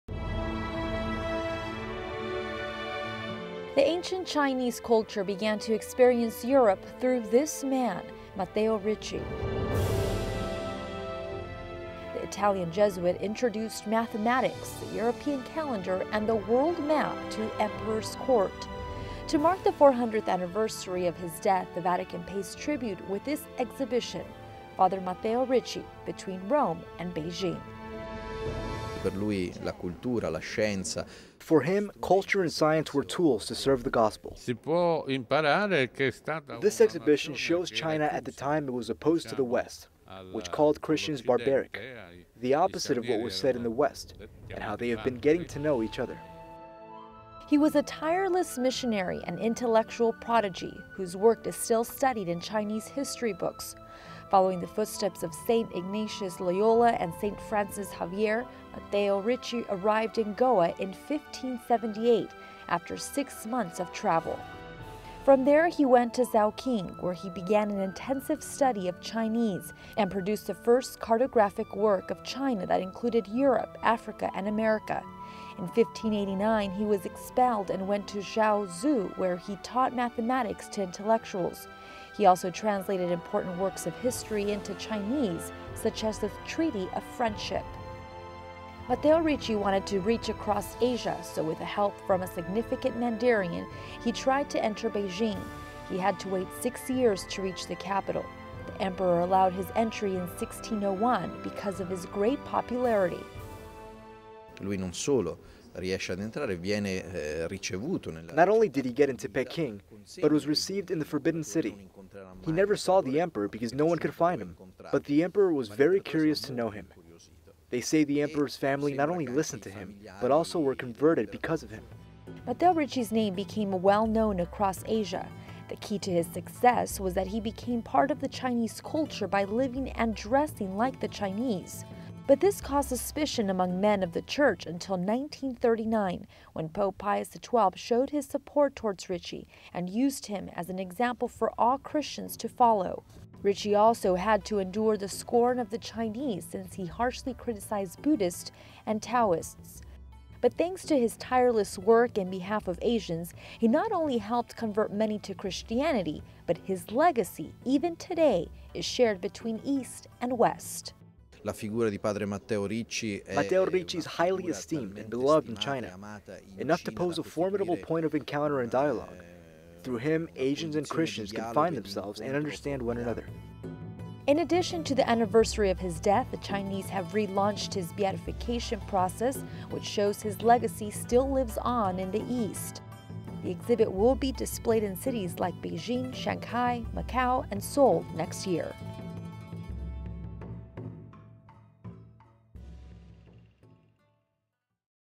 Matteo Ricci, an exhibition in the Vatican
The ancient Chinese culture began to experience Europe through this man: Matteo Ricci. The Italian Jesuit introduced mathematics, the European calendar and the world map to the Emperors Court. To mark the 400th anniversary of his death, the Vatican pays tribute with this exhibition: Father Matteo Ricci 'Between Rome and Beijing'.

Mons Claudio Giuliodori
Bishop of Macerata
"For him, culture and science were tools to serve the Gospel."

Giovanni Morello
Director
"This exhibition shows China at the time it was  opposed to the West, which called Christians barbaric, the opposite of what was said in the West, and how they have been getting to know each other."

He was a tireless missionary and intellectual prodigy whos work is still studied in Chinese history books. Following the footsteps of St. Ignatius Loyola and St. Francis Xavier, Matteo Ricci arrived in Goa in 1578, after six months of travel.

From there he went to Zhaoqing where he began an intensive study of Chinese and produced the first cartographic work of China that included Europe, Africa and America. In 1589 he was expelled and went to Shaozhou, where he taught mathematics to intellectuals. He also translated important works of history into Chinese such as the 'Treaty of Friendship'.

Matteo Ricci wanted to reach across Asia, so with help from a significant Mandarin he tried to enter Beijing. He had to wait six years to reach the capital. The emperor allowed his entry in 1601 because of its great popularity.

Mons Claudio Giuliodori
Bishop of Macerata
"Not only did he get into Peking, but was received in the Forbidden City. He never saw the emperor, because none could be found. But the emperor was very curious to know him. They say the emperor's family not only listened to him but also were converted because of him ".

Matteo Riccis name became well known across Asia. The key to his success was that he became part of Chinese culture by living and dressing like the Chinese. But this caused suspicion among men of the Church until 1939 when Pope Pius XII showed his support towards Ricci and used him as an example for all Christians to follow.

Ricci also had to endure the scorn of the Chinese, since he harshly criticized Buddhists and Taoists.

But thanks to his tireless work in behalf of  Asians, he not only helped convert many to Christianity, but his legacy, even today, is shared between East and West.

Mons Claudio Giuliodori
Bishop of Macerata
"Matteo Ricci is highly esteemed and beloved in China. Enough to pose a formidable point of encounter and dialogue. Through him, Asians and Christians can find themselves and understand one another.

In addition, to the anniversary of his death, the Chinese have re-launched his beautification process which shows his legacy still lives on in the East.

The exhibit will be displayed in cities like Beijing, Shanghai, Macao and Seoul next year.

PVB/MC-JM .